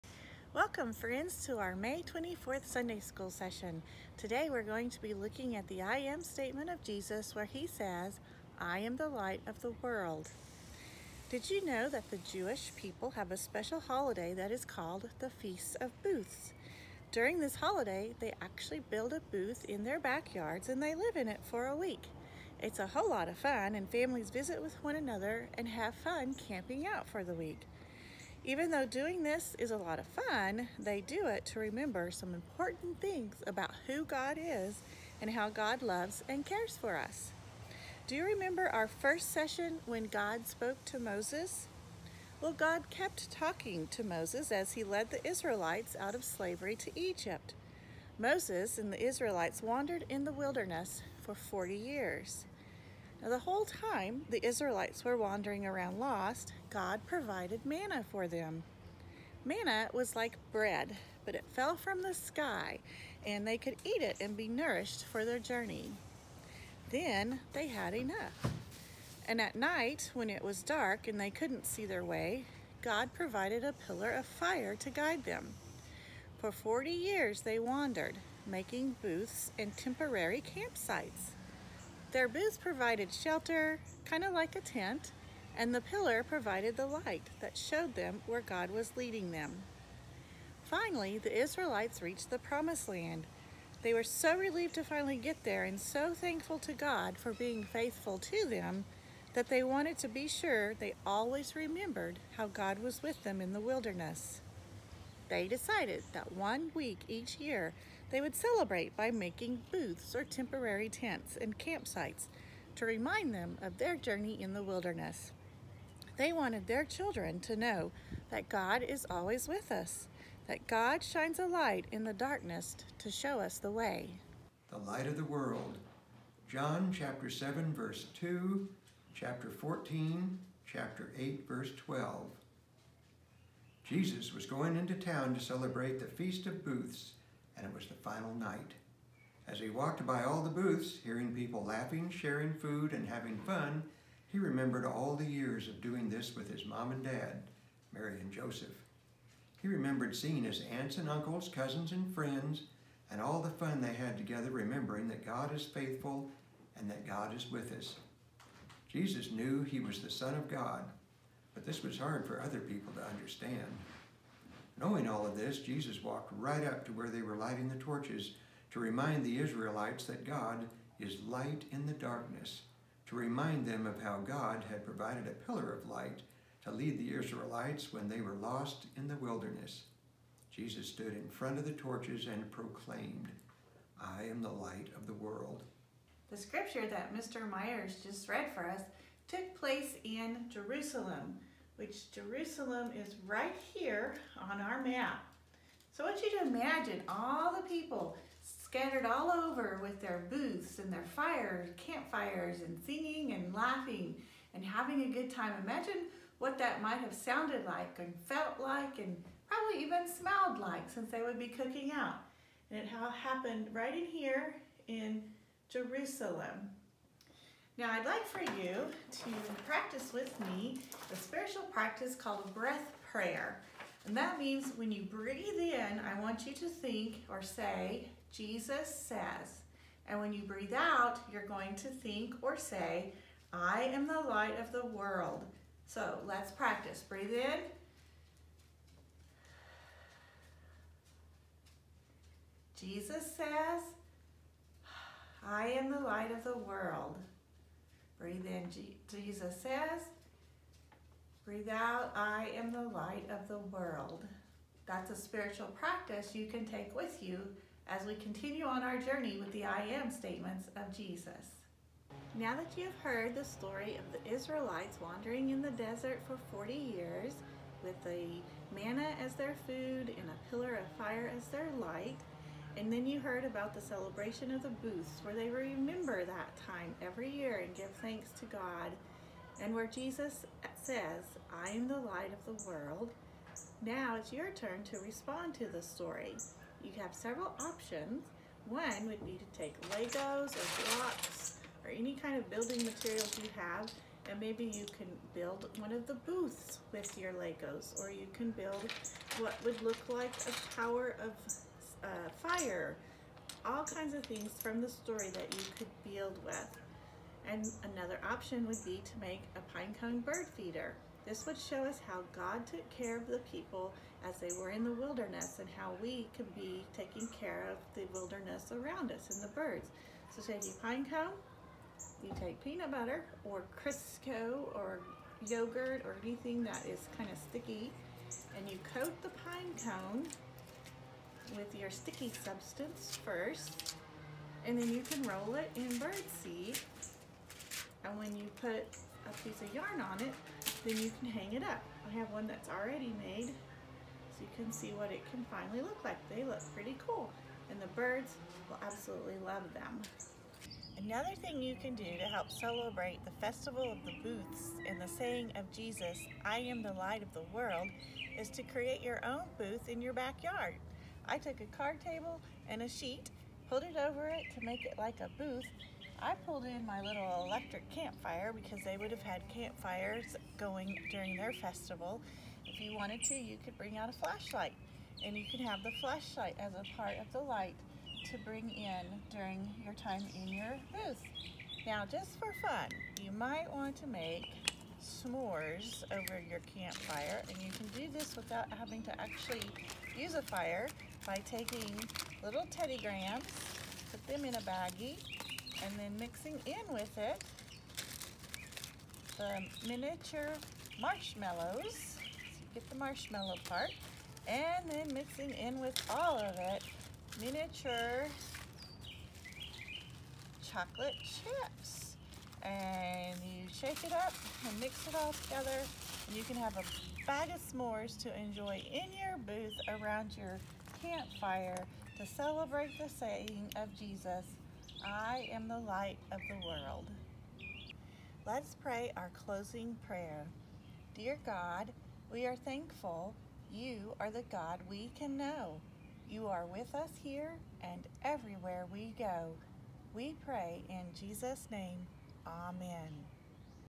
May 24 Elementary Sunday School: The Light of the World
Rev. EvaMarie Campbell, Associate Minister at Boston Avenue UMC, shares about Jesus saying, I am the light of the world.